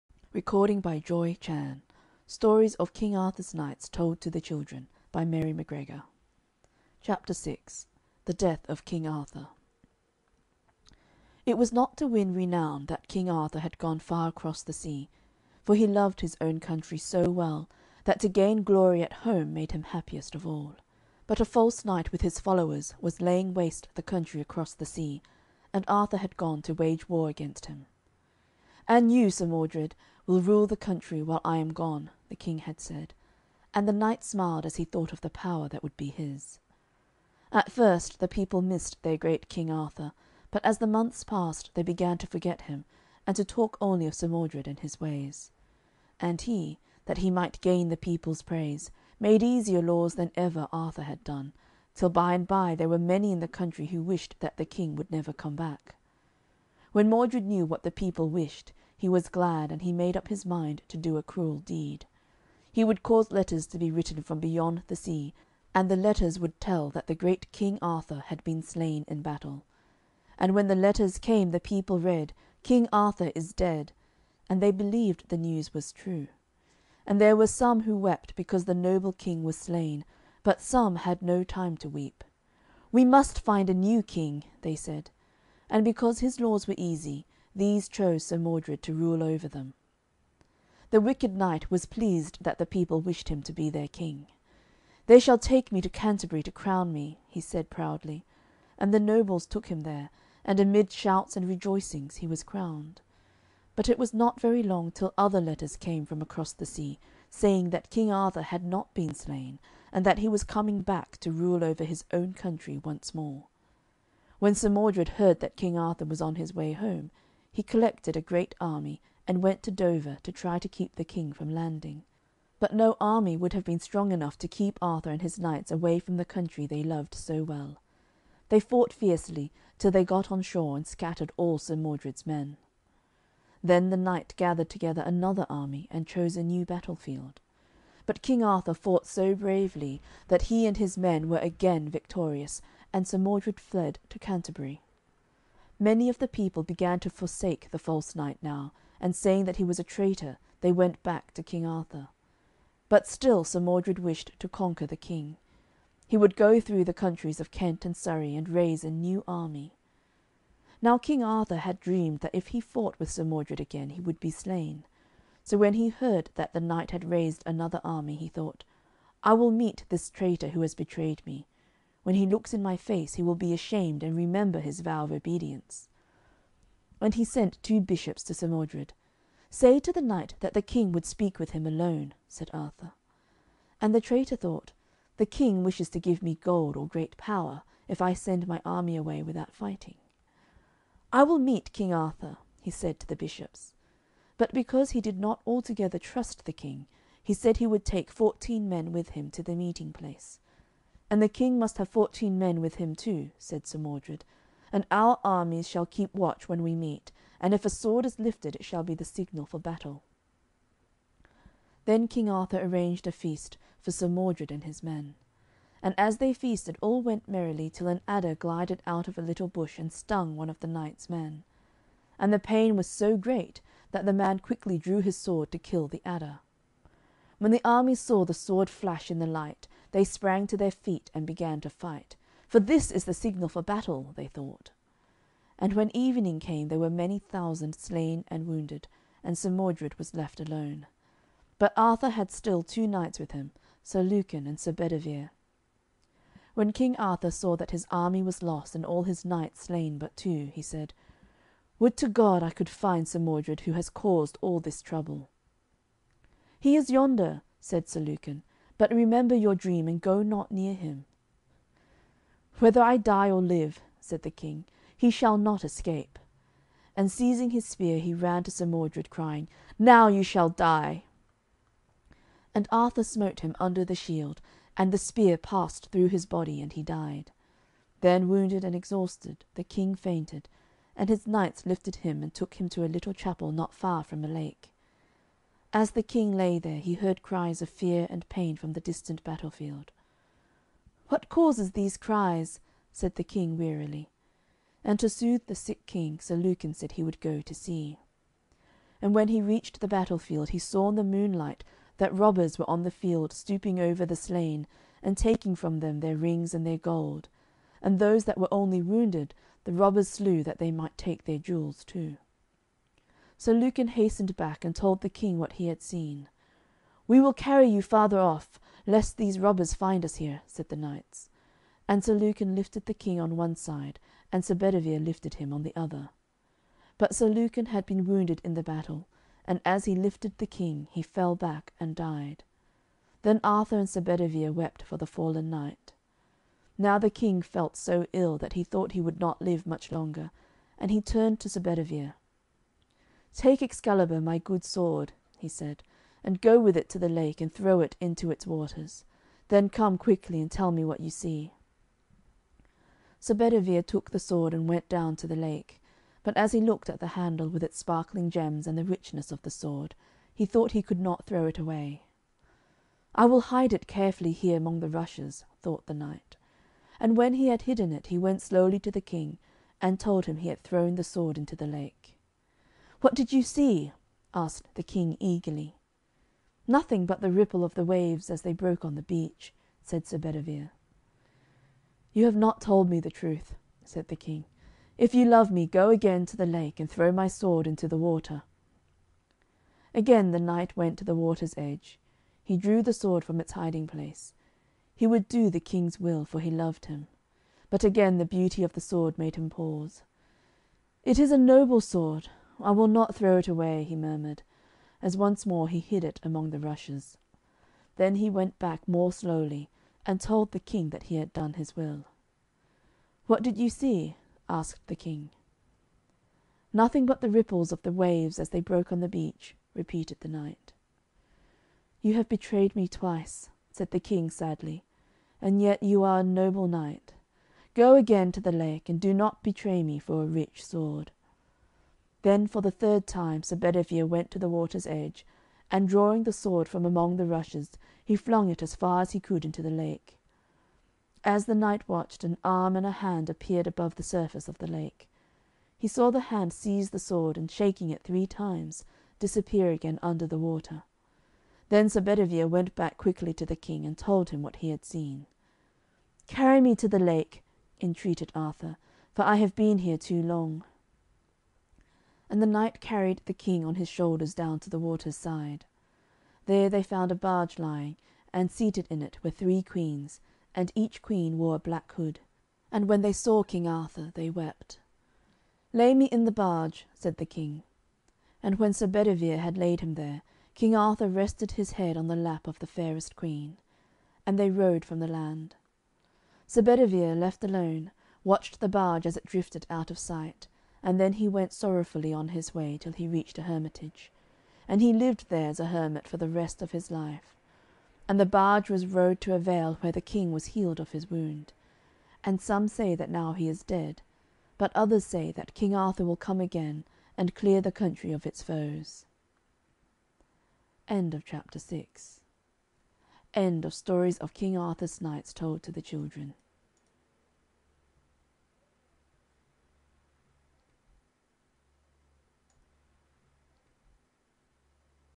King Arthur and His Knights of the Round Table by Roger Lancelyn Green COMPLETE Audiobook - Pt 6
King Arthur and His Knights of the Round Table is a retelling of the Arthurian legends, principally Thomas Malory's Le Morte d'Arthur, by Roger Lancelyn Green. It was intended for a child audience.